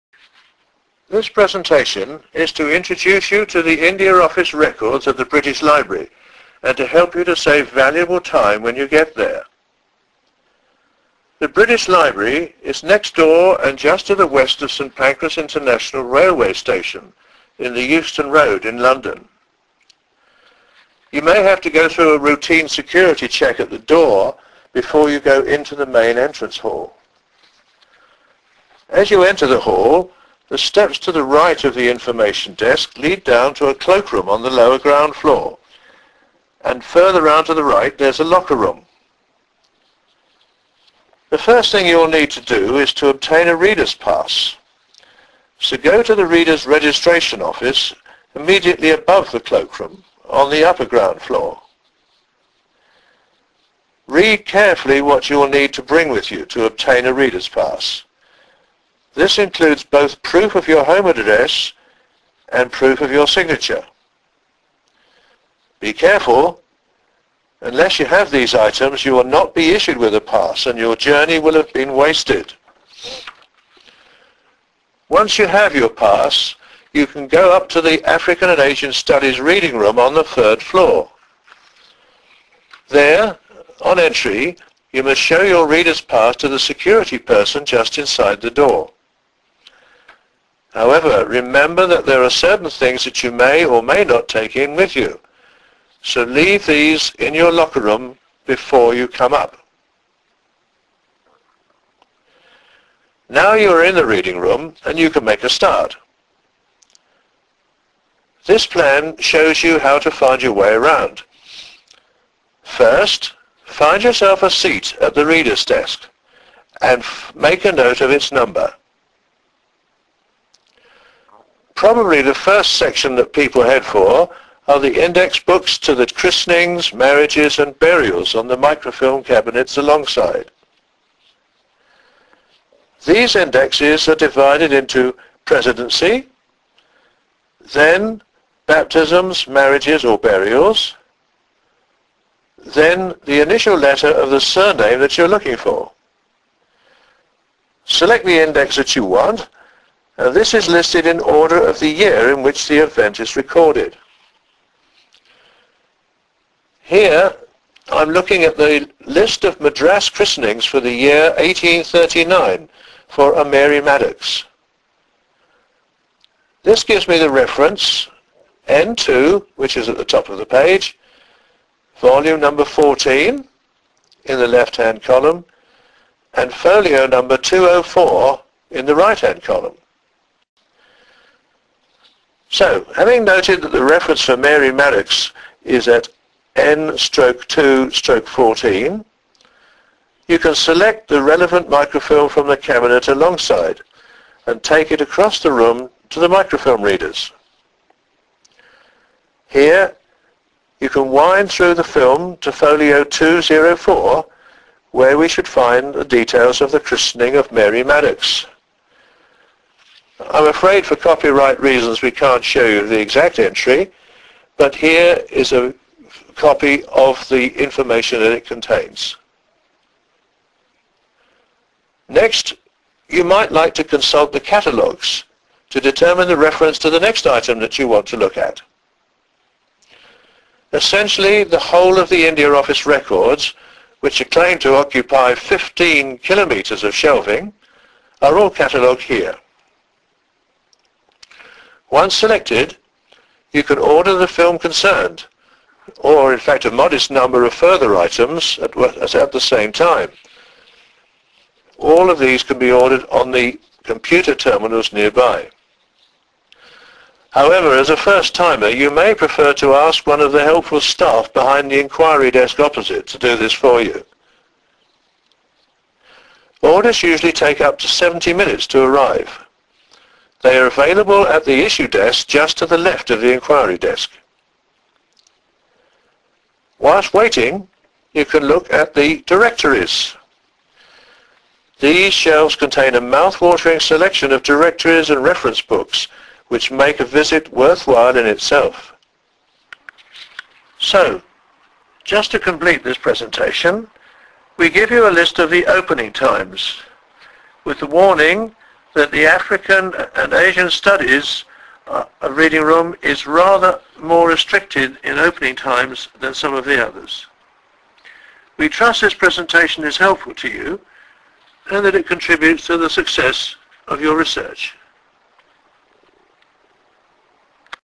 Advice For Your First Visit to the APAC Reading Rooms
Advice from the Chairman of the Families In British India Society, Peter Bailey, on your first visit to the Asia Pacific and Africa Collection Reading Rooms at the British Library to research your family history in British India.